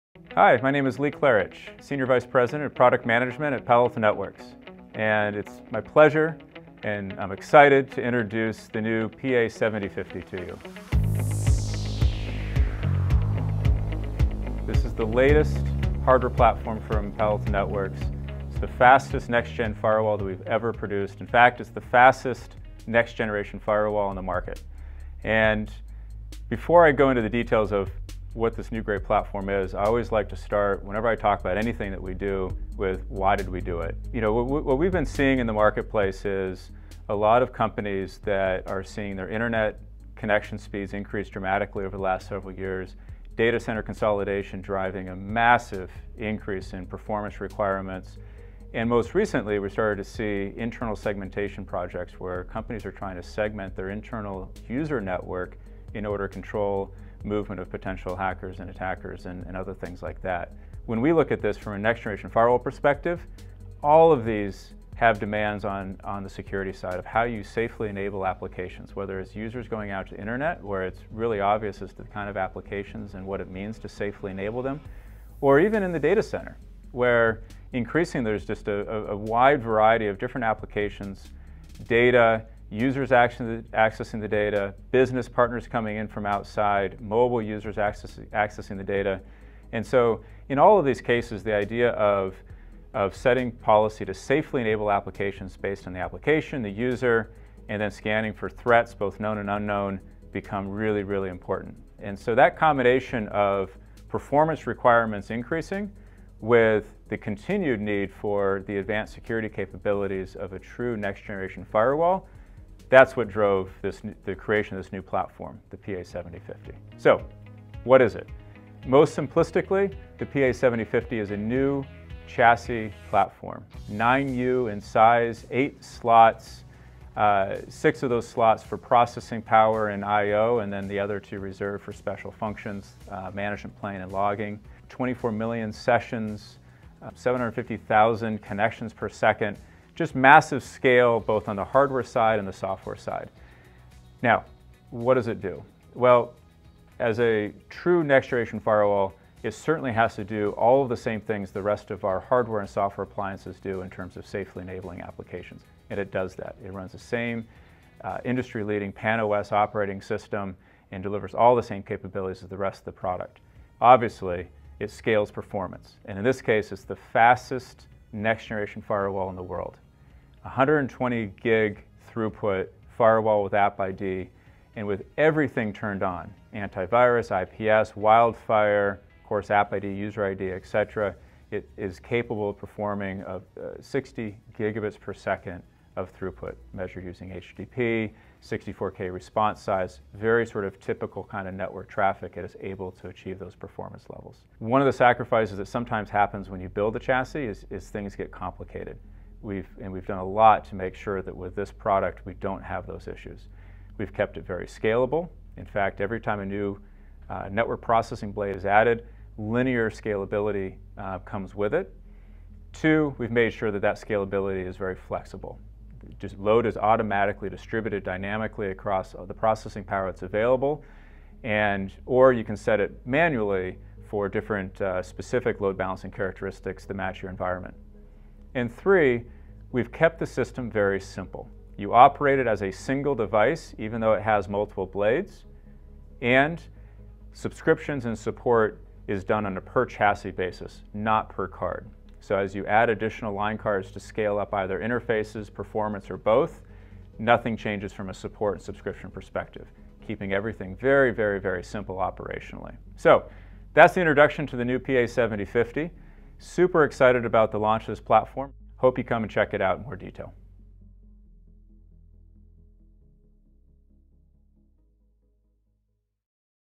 Palo Alto PA 7050
Hear first hand from Lee Klarich, SVP of Products at Palo Alto, discuss what makes the Palo Alto PA-7050 The Fastest Next-Generation Firewall.

For more details on the Palo Alto PA-7050 and to arrange a demo click or copy and paste the following link to chat with a Prodec Network Palo Alto PA-7050 specialist